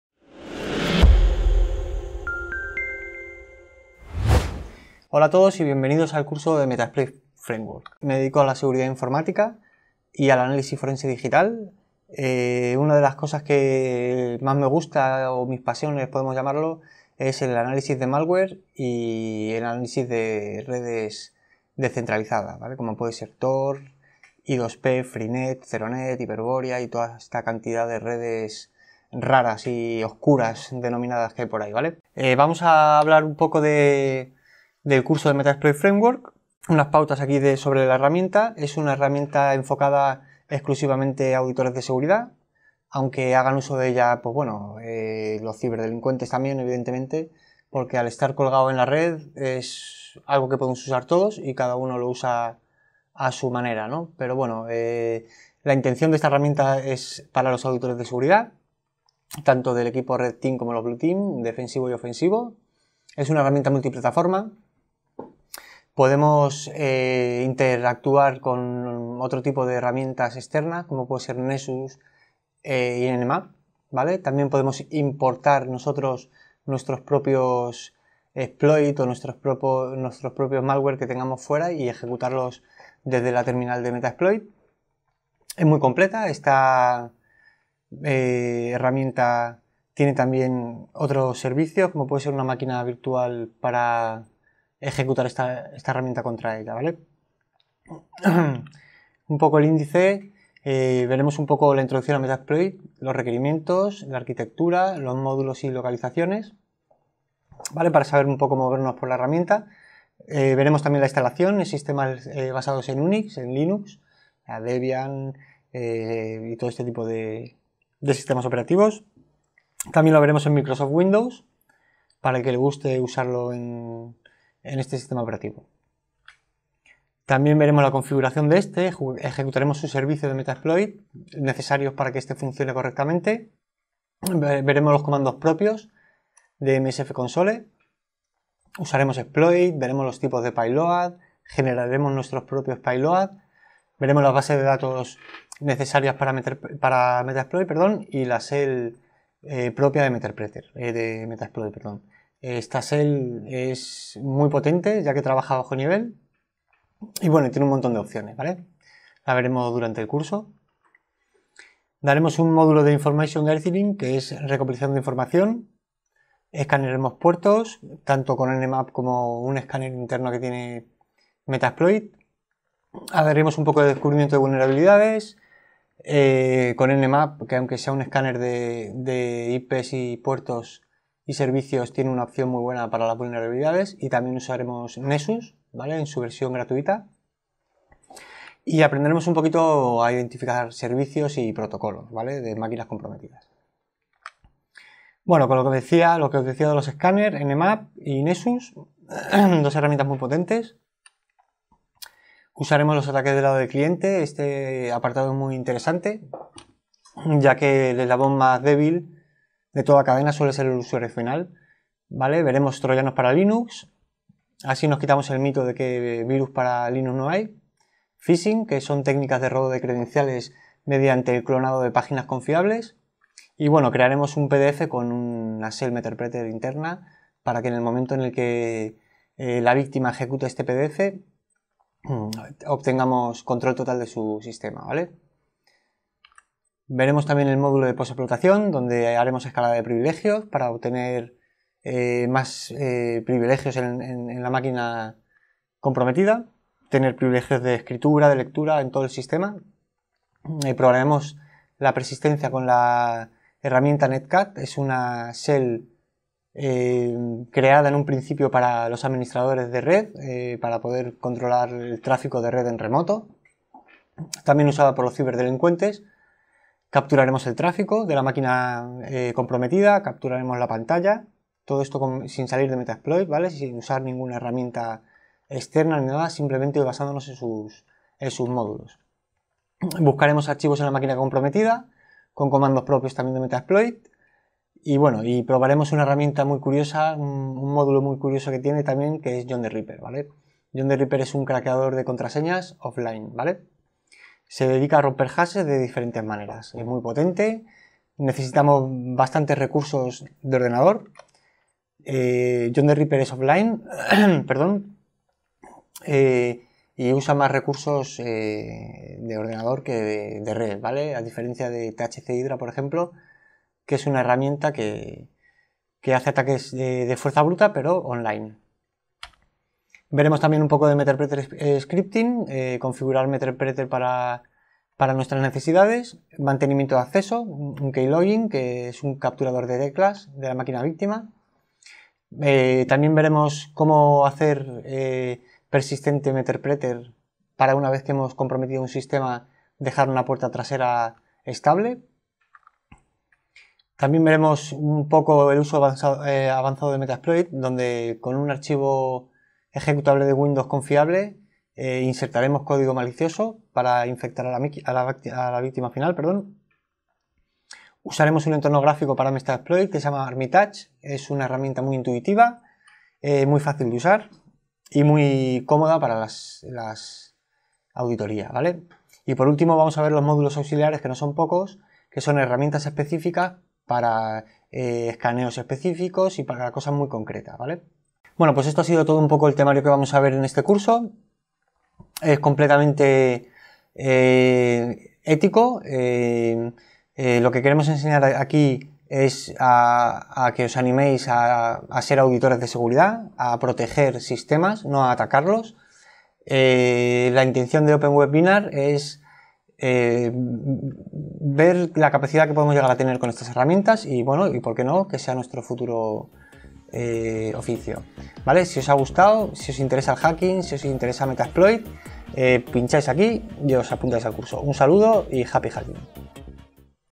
CURSO METASPLOIT FRAMEWORK
Aprende a usar Metasploit, el framework de herramientas más usado para hacer pentesting con este curso totalmente en español.

Recomendamos tener conocimientos básicos de Linux y de uso de máquinas virtuales.

¿Qué vas aprender con este curso?

► Qué es un exploit
► Cómo funciona un exploit
► Qué es un Payloads
► Opciones de Payloads
► Técnicas e importancia del Footprinting de redes auditadas
► Descubrir vulnerabilidades
► Descartar falsos positivos.
► Herramientas más usadas de metaploit
► Usar escaners de servicios y puertos
► Protecciones activas y pasivas